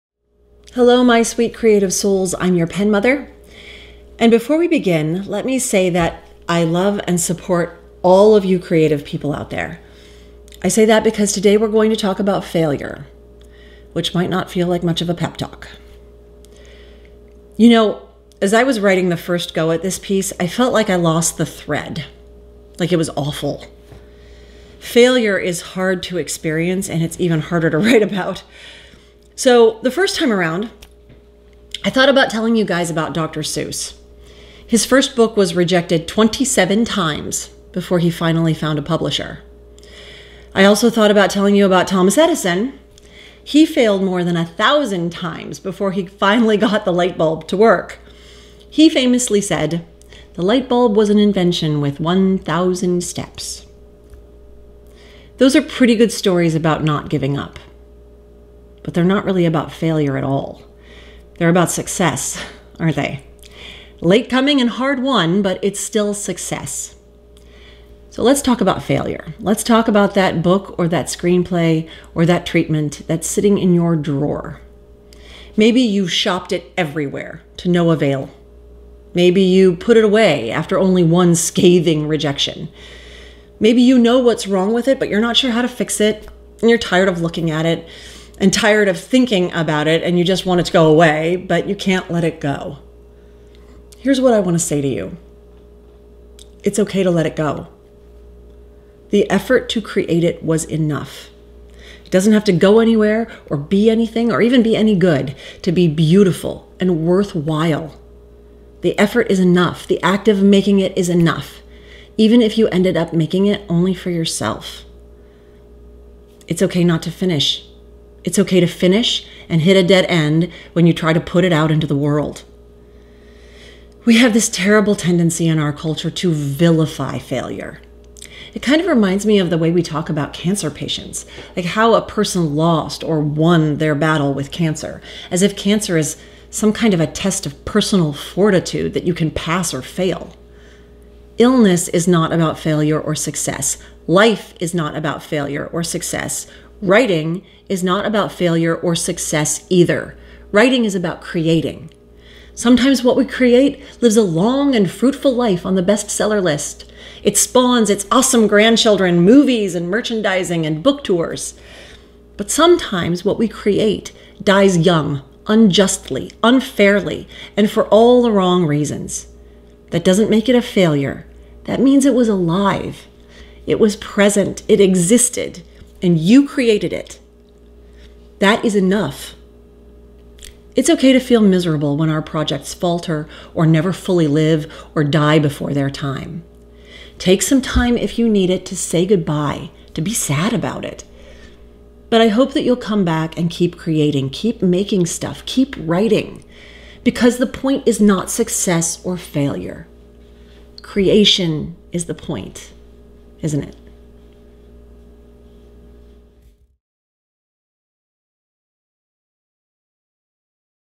Pen Mother: When We Fail
Sometimes things don't work out the way we planned or hoped. Sometimes even our best efforts fall short. It happens to all of us. It's okay. It really is okay.